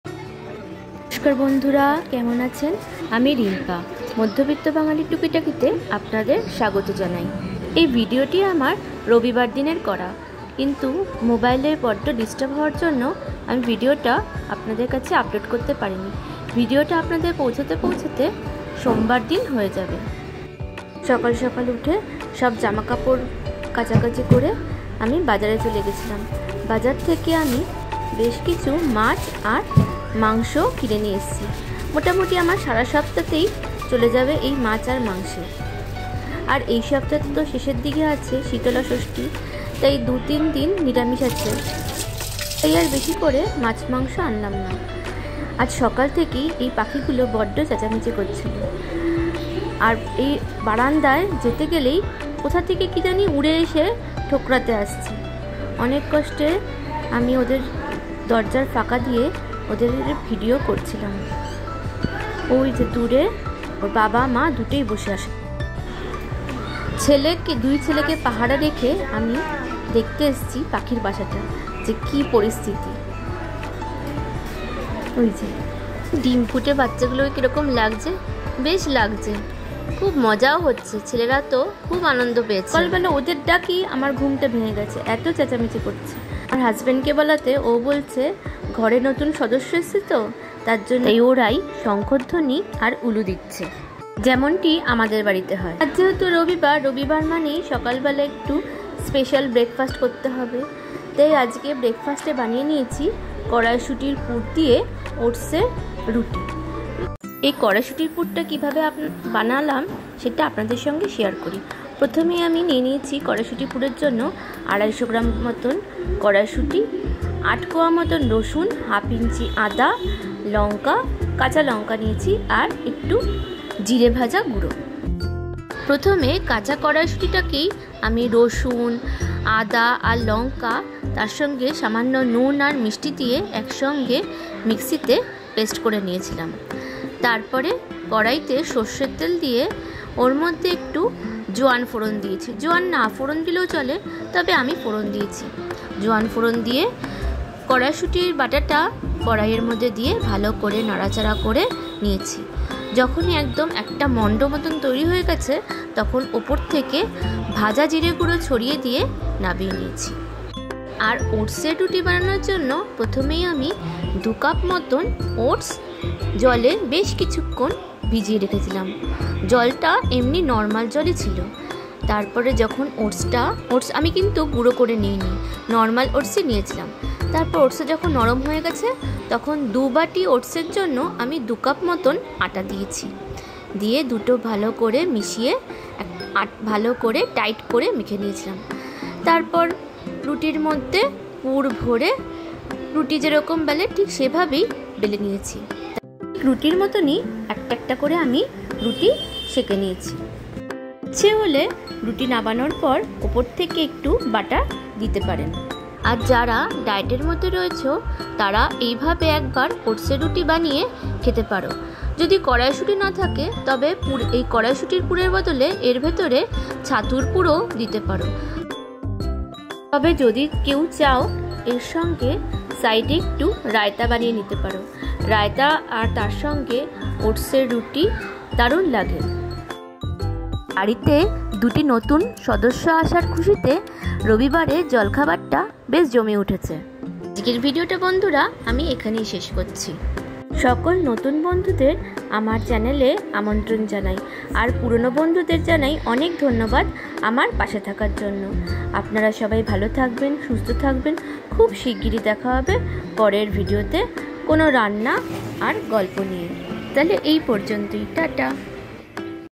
baniye fellam motorsuti-oats er rooti/barite notun sodosso elo😘🤩❣️🤗 oats_ruti_recipe red-whiskered
baniye fellam motorsuti-oats er rooti/barite notun sodosso elo😘🤩❣️❣️🤗
red-whiskeredbulbul